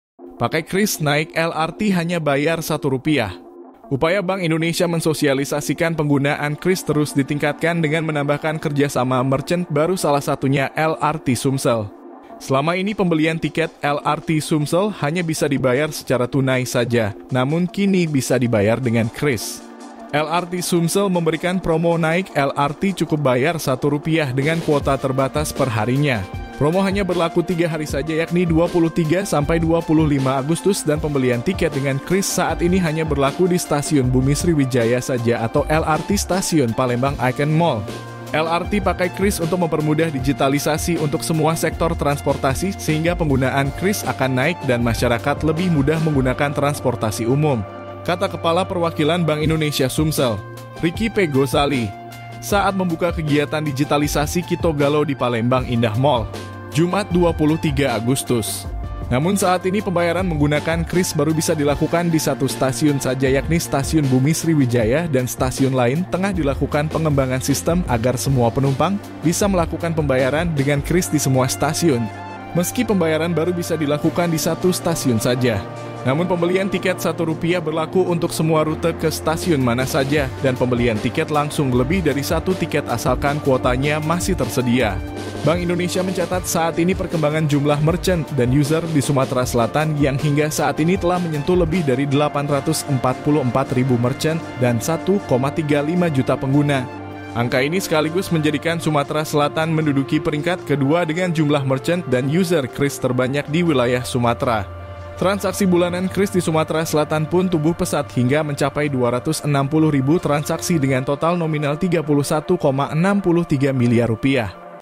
Pakai QRIS, Naik LRT Hanya Bayar Rp 1
Upaya Bank Indonesia mensosialisasikan penggunaan QRIS terus ditingkatkan dengan menambahkan kerjasama merchant baru salah satunya LRT Sumsel. Selama ini pembelian tiket LRT Sumsel hanya bisa dibayar secara tunai saja namun kini bisa dibayar dengan QRIS.